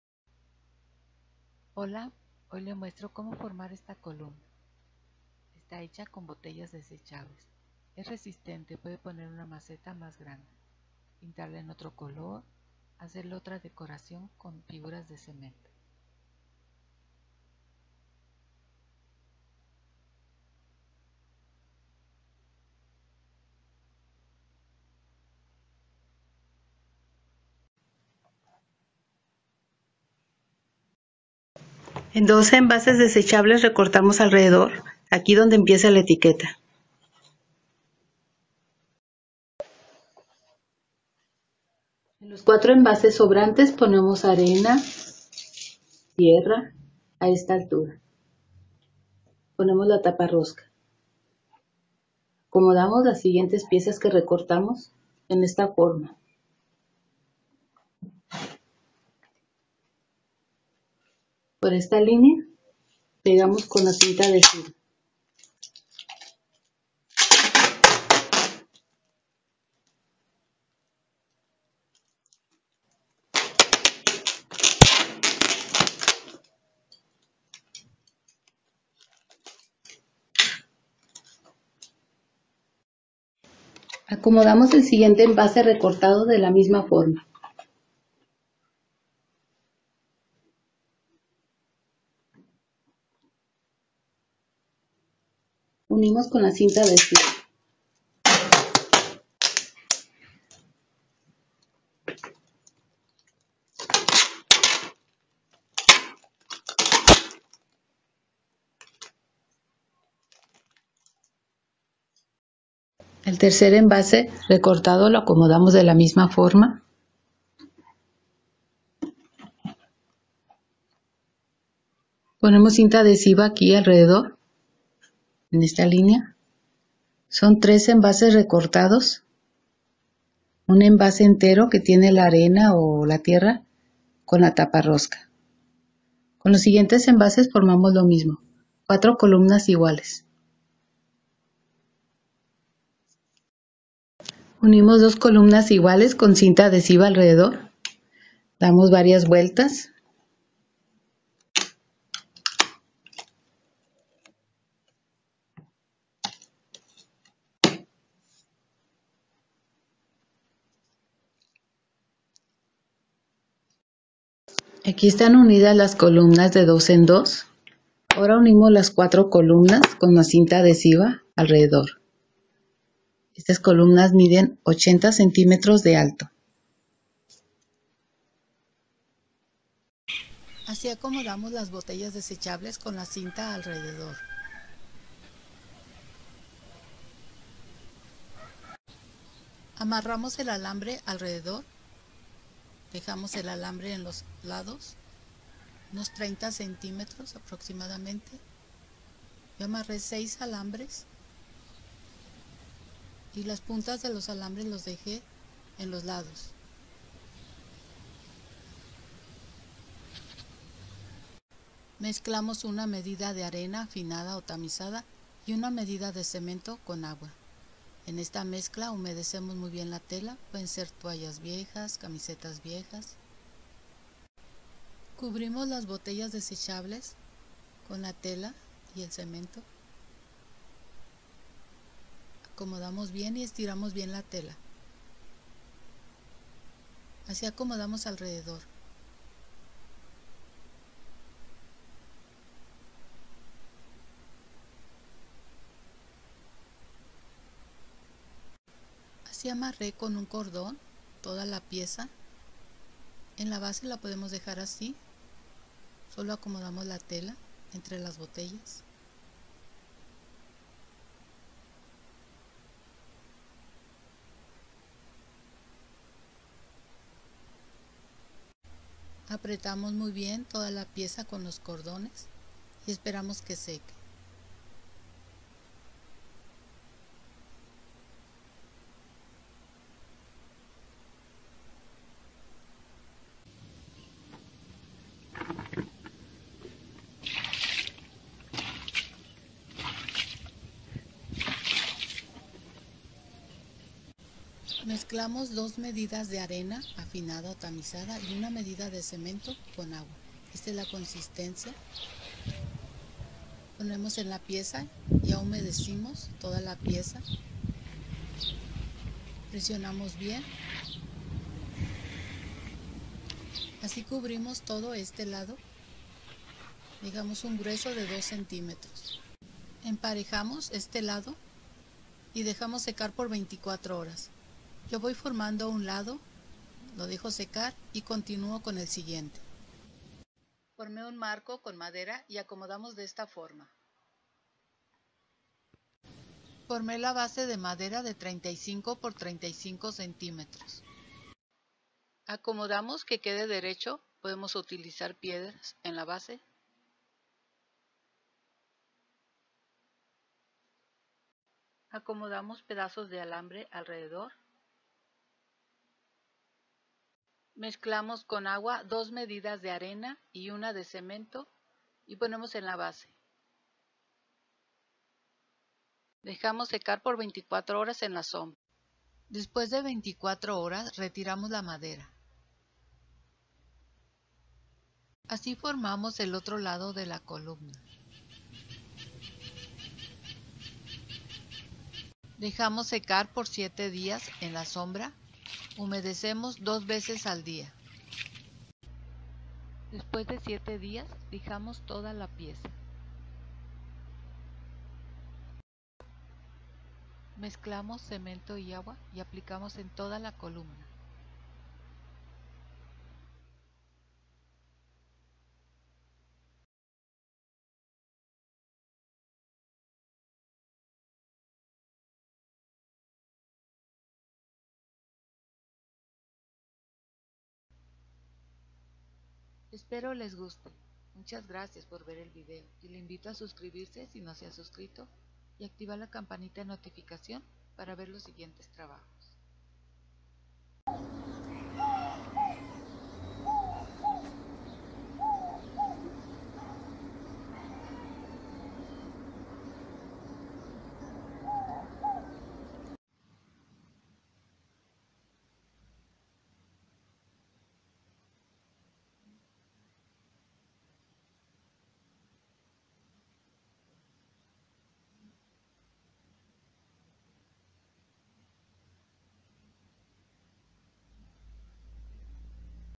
COMO HACER COLUMNA DE CEMENTO Y BOTELLAS DESECHABLES
FACIL Y RESISTENTE ! MATERIALES   cemento,arena afinada o tamizada,  agua, telas viejas, 16 botellas desechables grandes de 2 litros y medio, alambre y tablas de 3 centimetros de ancho por 30 centimetros,  para hacer el marco